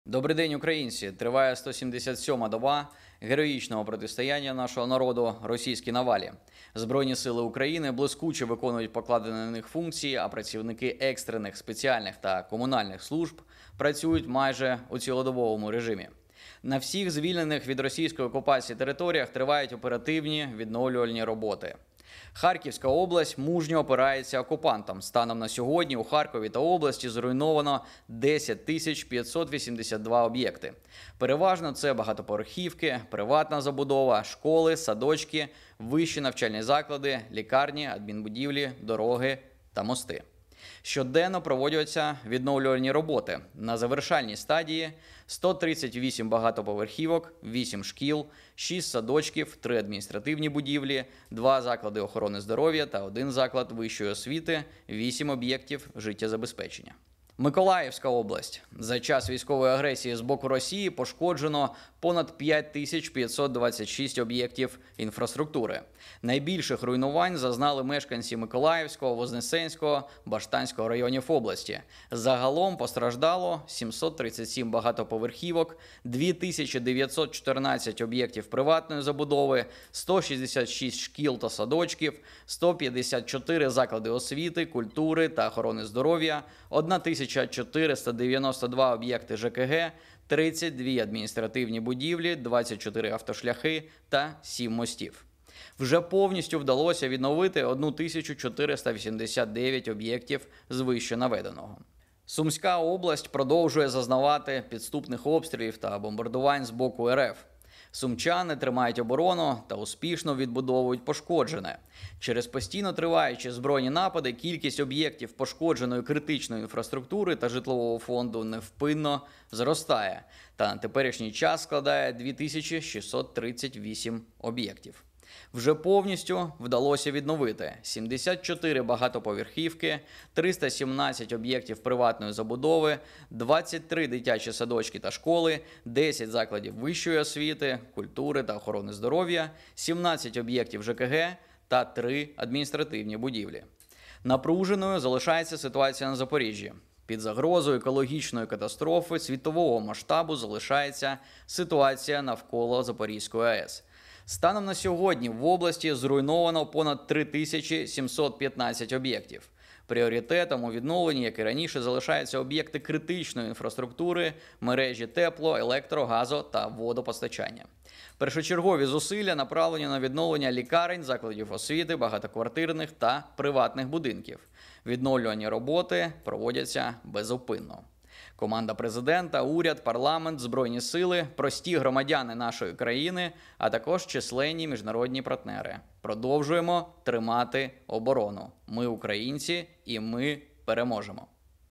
Брифінг заступника Керівника ОПУ Кирила Тимошенка
Добрий день, українці!
Триває 177 доба героїчного протистояння нашого народу російській навалі. Збройні сили України блискуче виконують покладені на них функції, а працівники екстрених, спеціальних та комунальних служб працюють майже у цілодобовому режимі.

На всіх звільнених від російської окупації територіях тривають оперативні відновлювальні роботи.

Харківська область мужньо опирається окупантам. Станом на сьогодні  у Харкові та області зруйновано 10 582 об’єкти. Переважно це багатоповерхівки, приватна забудова, школи, садочки, ВНЗ, лікарні, адмінбудівлі, дороги та мости.

Щоденно проводяться відновлювальні роботи.
На завершальній стадії:
• 138 багатоповерхівок
• 8 шкіл
• 6 садочків
• 3 адміністративні будівлі
• 2 заклади охорони здоров’я та 1 заклад вищої освіти
• 8 об’єктів життєзабезпечення

Миколаївська область. За час військової агресії з боку росії пошкоджено понад 5 526 об’єктів інфраструктури. Найбільших руйнувань зазнали мешканці Миколаївського, Вознесенського, Баштанського районів області. Загалом постраждало 737 багатоповерхівок, 2 914 об’єктів приватної забудови, 166 шкіл та садочків, 154 заклади освіти, культури та охорони здоров’я, 1 492 об’єкти ЖКГ, 32 адміністративні будівлі, 24 автошляхи та 7 мостів. Вже повністю вдалося відновити 1 489 об’єктів з вищенаведеного.

Сумська область продовжує зазнавати підступних обстрілів та бомбардувань з боку рф.  Сумчани тримають оборону та успішно відбудовують пошкоджене. Через постійно триваючі збройні напади, кількість об’єктів пошкодженої критичної інфраструктури та житлового фонду невпинно зростає та на теперішній час складає 2 638 об’єктів.
Вже повністю вдалося відновити: 74 багатоповерхівки, 317 об’єктів приватної забудови, 23 дитячі садочки та школи,  10 закладів вищої освіти, культури та охорони здоров’я, 17 об’єктів ЖКГ та 3 адміністративні будівлі.

Напруженою залишається ситуація на Запоріжжі. Під загрозою екологічної катастрофи світового масштабу залишається ситуація навколо Запорізької АЕС. Станом на сьогодні в області зруйновано понад 3 715 об’єктів. Пріоритетом у відновленні, як і раніше, залишаються об’єкти критичної інфраструктури, мережі тепло-, електро-, газо-, водопостачання. Першочергові зусилля направлені для відновлення лікарень, закладів освіти, багатоквартирних та приватних будинків. Відновлювальні роботи проводяться безупинно.

Команда Президента, Уряд, Парламент, Збройні Сили України, прості громадяни нашої країни, а також численні міжнародні партнери –  продовжуємо тримати оборону!
Ми – українці!
Ми – переможемо!